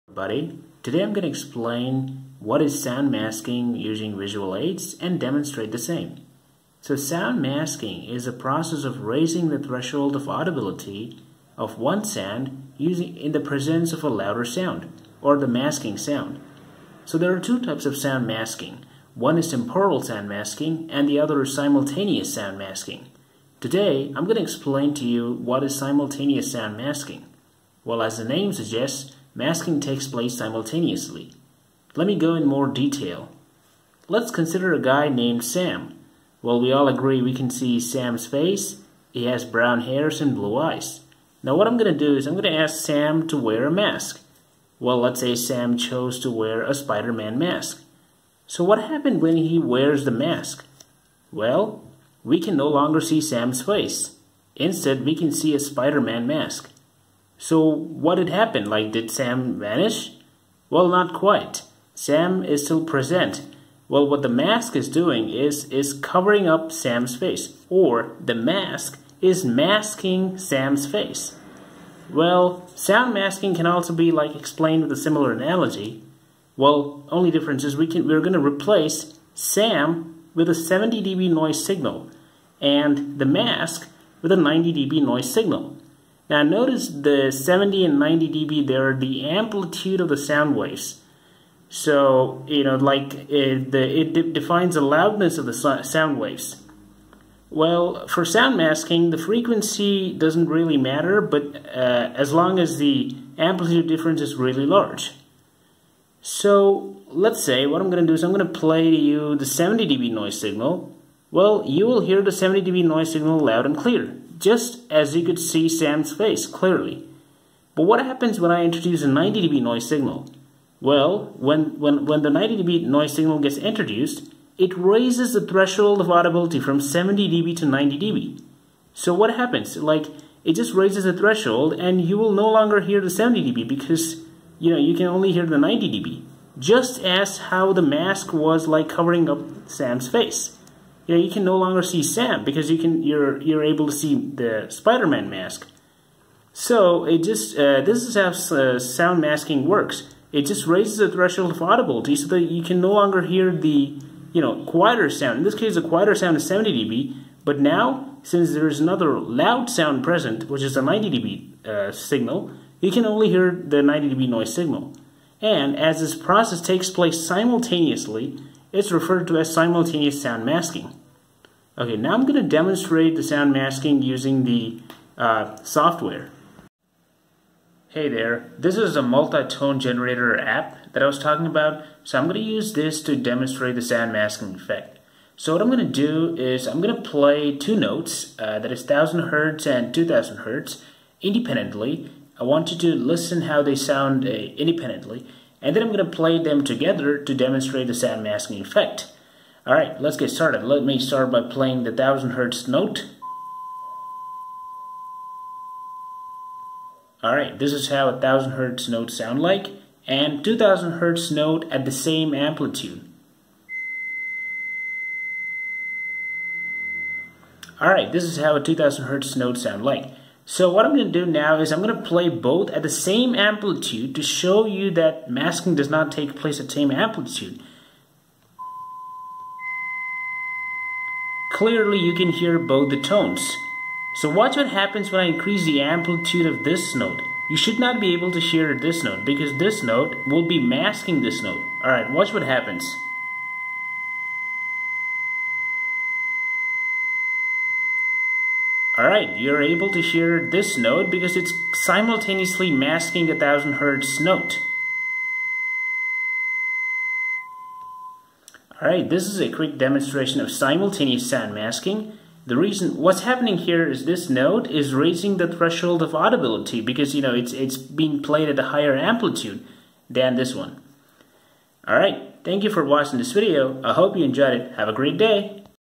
Simultaneous sound masking explained and demonstrated
Sound masking is the process of raising the threshold of audibility in the presence of a louder (masker) sound. There are two types of sound masking i.e. simultaneous sound masking and temporal sound masking. In this video I have explained simultaneous sound masking with visual aids. I have also demonstrated the same by playing two notes  i.e. 1000Hz note and 2000Hznote at different amplitudes. The 2000Hz note is played at a higher amplitude than the 1000Hz note thereby masking it.